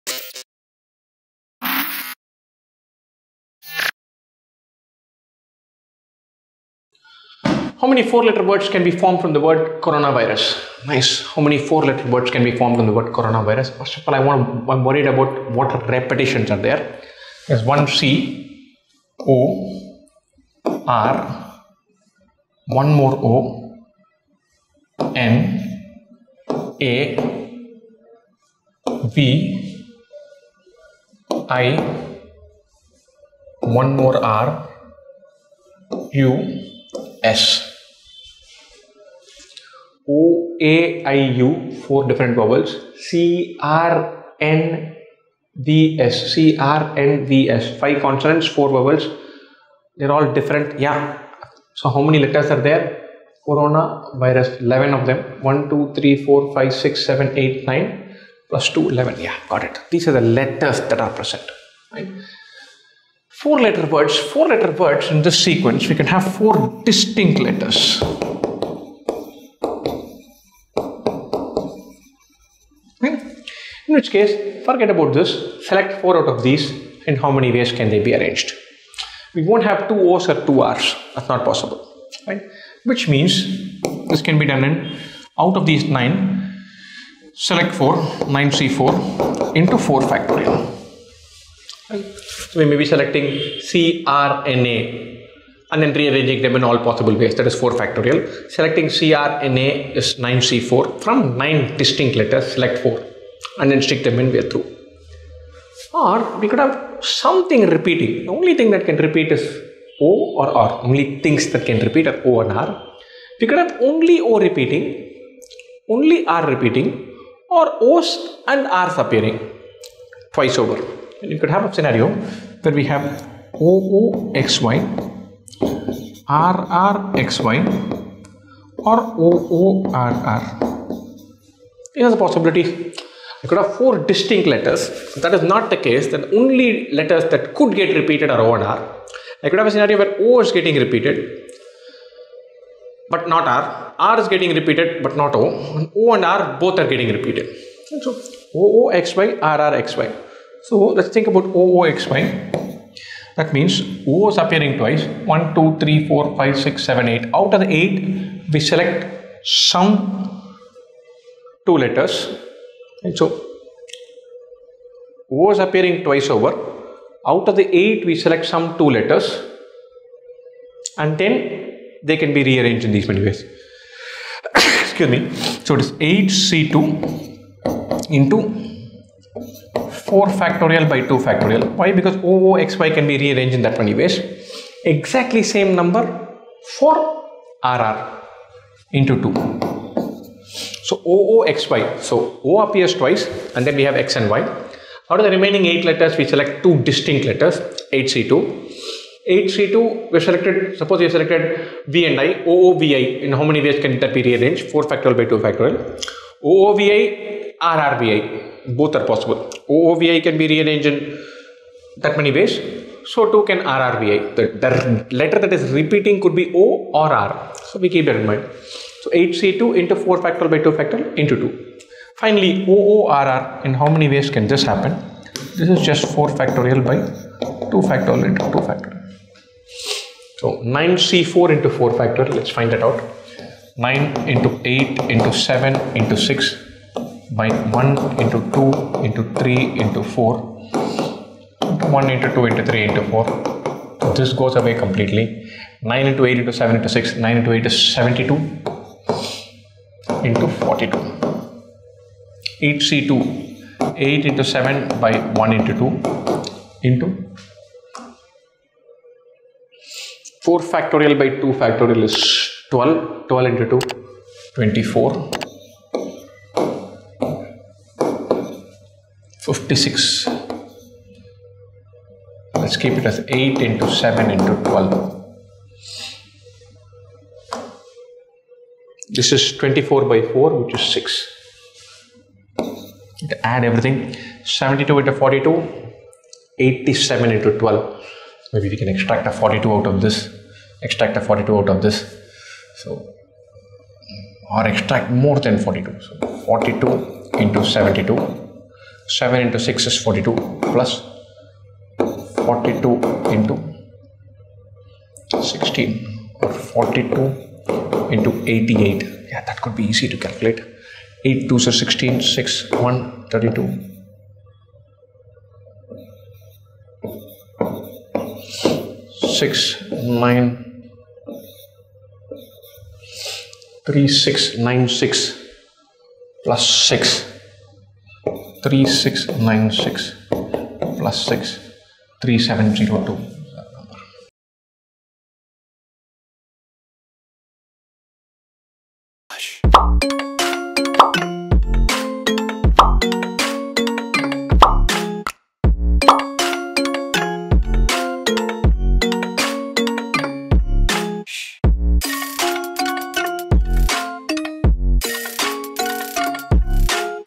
IIFT 2021 Quant Solutions | Four Letter Words | IIFT 2022 Prep | 2IIM CAT Preparation
Hey Folks!
Now that CAT is over, it's a good time to start preparing for IIFT. The best place to start preparing for IIFT quant is by solving previous year papers.

2IIM is back with another set of quant solutions and this time we're attacking IIFT 2021.

Best Wishes!